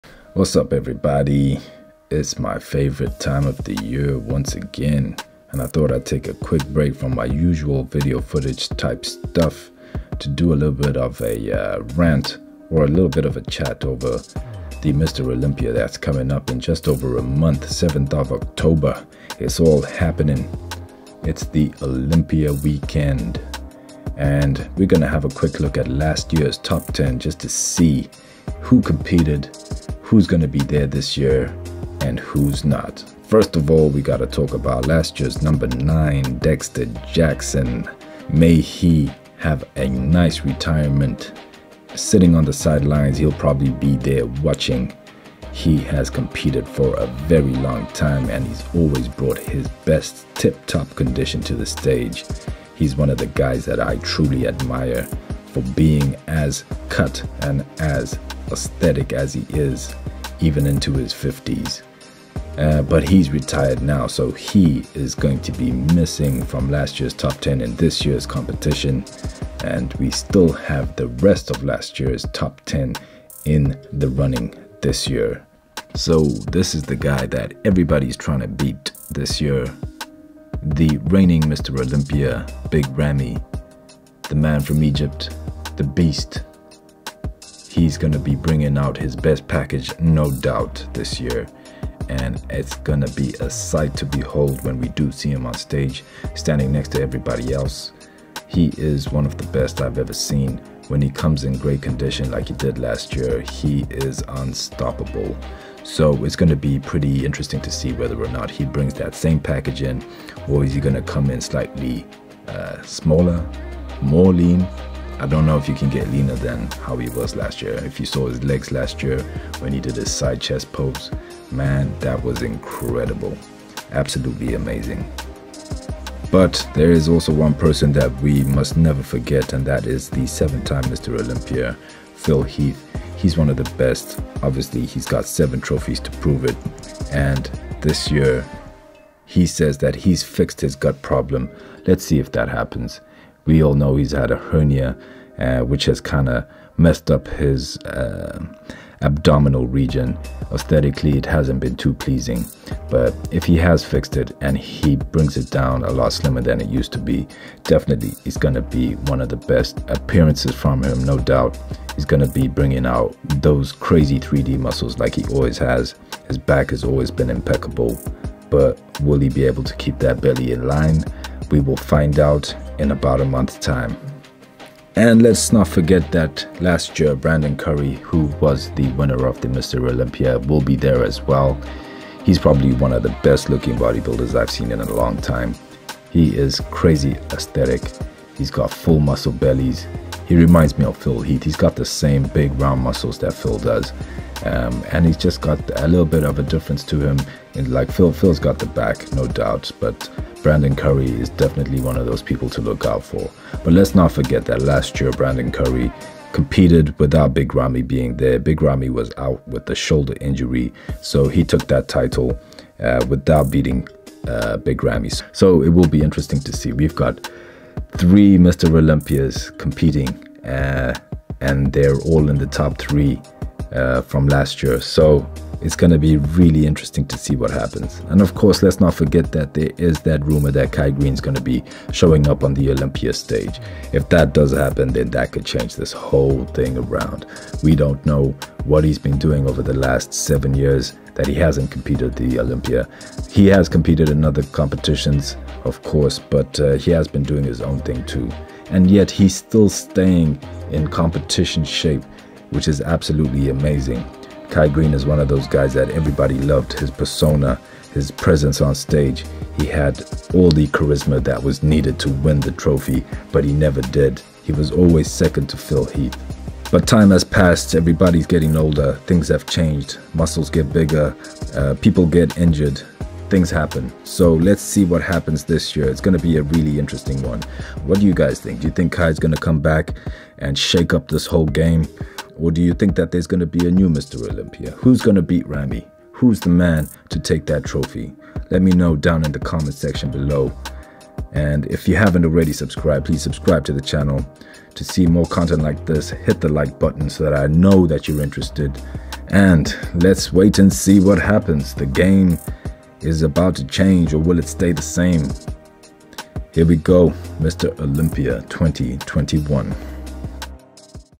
Who Will Win Mr Olympia 2021? Top 10
In this video I talk about the upcoming Olympia weekend top 10 contestants from last year. Who will win it this time around? Can Ramy retain the title?

Carnivor Shred